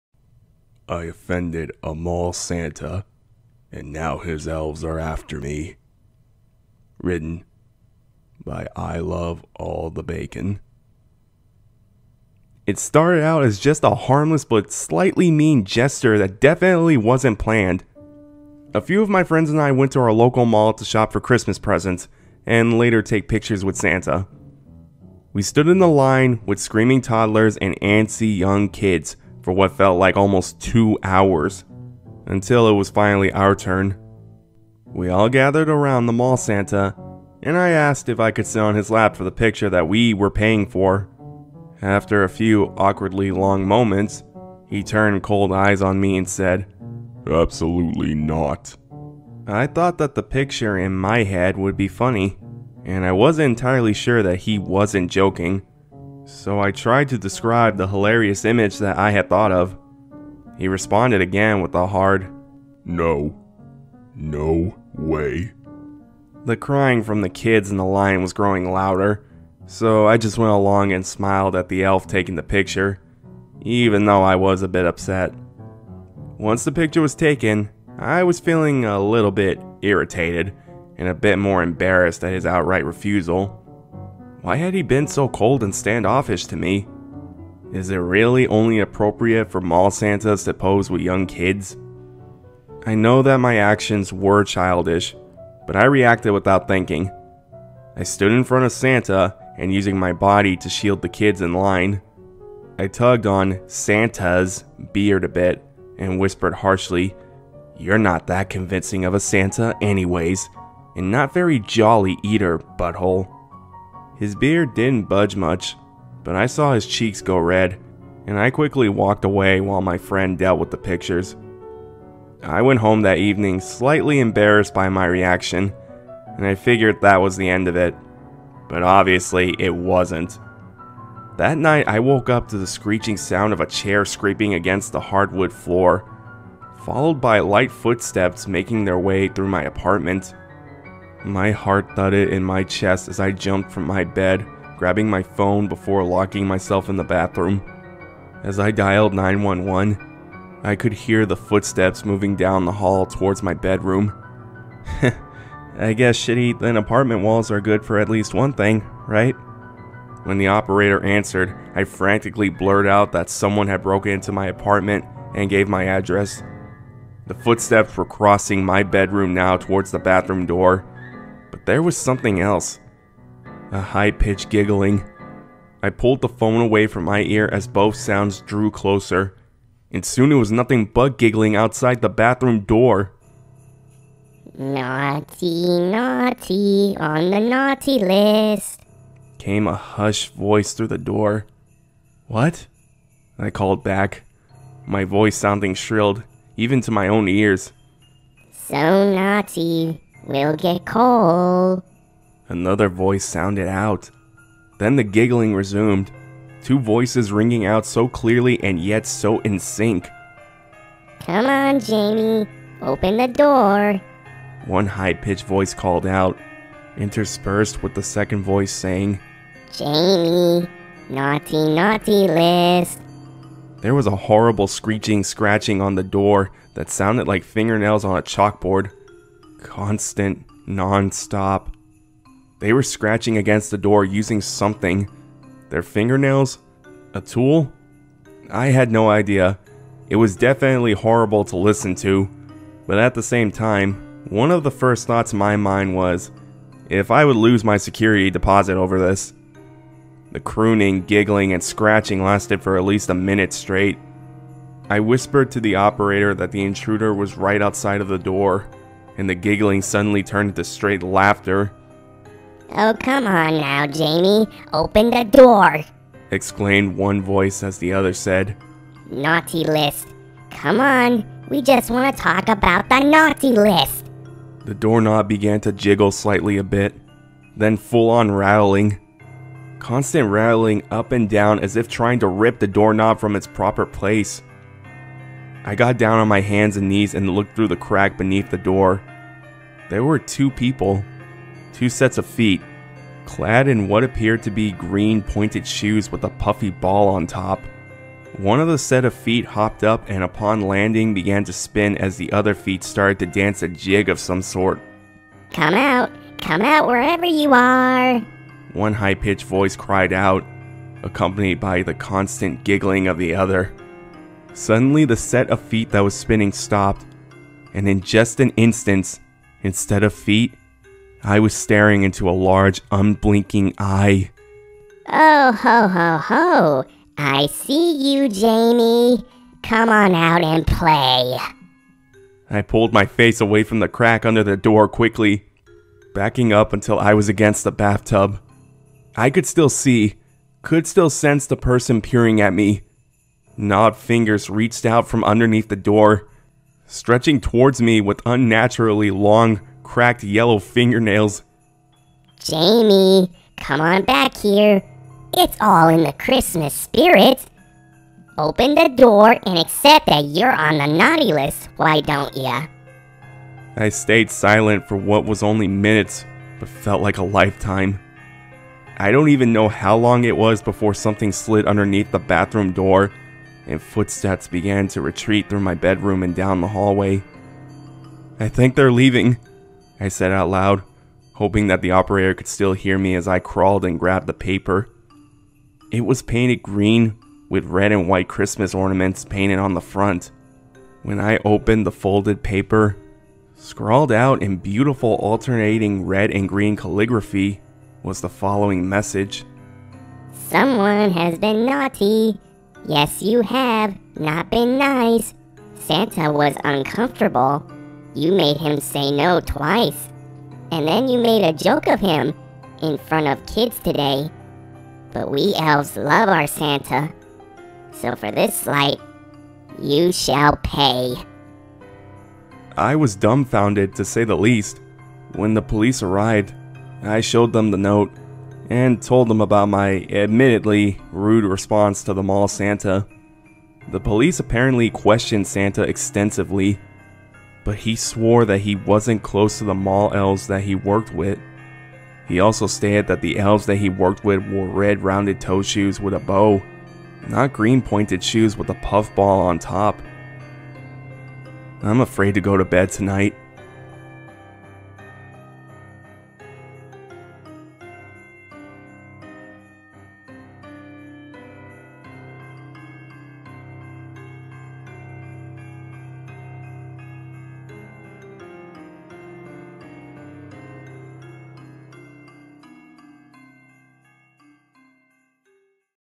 I offended a mall Santa and now his elves are after me - Creepypasta
Those mall Santa’s have a pretty tough job, don’t make it any tougher on them by being a smarta** otherwise those elves might take offense.

Author iloveallthebacon

Story link